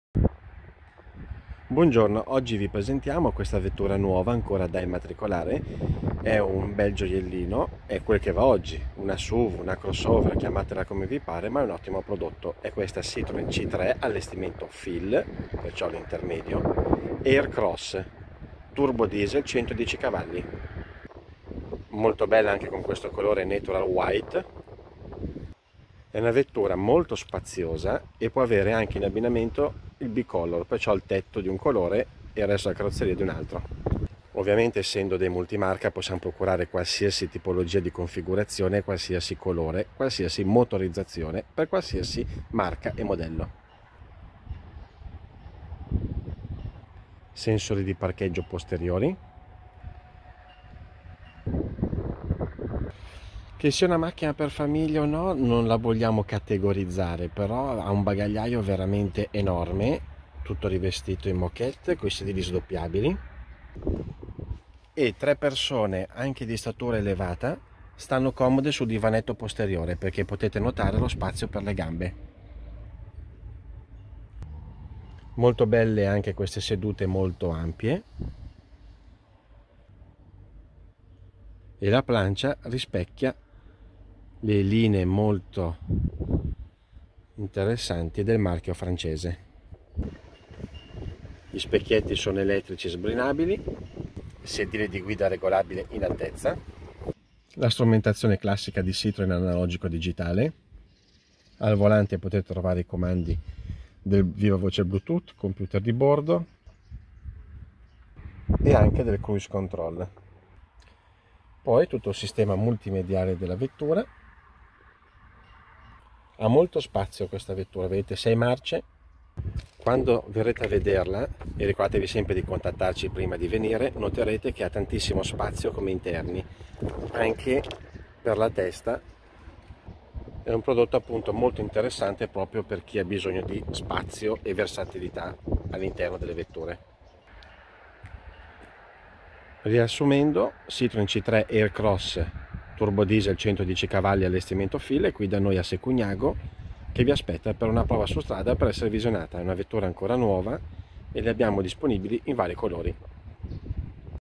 Citroen C3 Aircross 1.5 HDI 110cv FEEL
SUV Citroën C3 Aircross è l’unico SUV compatto del mercato che si ispira ai moderni bisogni del cliente. Al carattere protettivo dello stile esterno si aggiunge il fascino di una personalità distintiva, lo spazio e la versatilità da riferimento ed un comfort unico per la categoria. La dimostrazione del savoir-faire della Marca per design, abitabilità e benessere.

SUV Citroën C3 Aircross si distingue nell'universo dei SUV per la sua personalità forte, originale e fresca.

Il suo stile evoca robustezza e senso di sicurezza grazie al frontale rialzato, sottolineato dagli Chevron cromati che integrano i fari diurni a LED.

Nonostante le dimensioni compatte, gli ampi spazi interni garantiscono una abitabilità a bordo senza alcun compromesso.

A rimarcare la caratteristica da vero SUV compatto, la posizione di guida è piacevolmente alta e permette di dominare sulla strada.